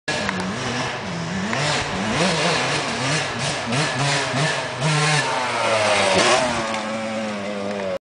jumping over trucks
my buddy jumping over my truck in the woods in dighton , ma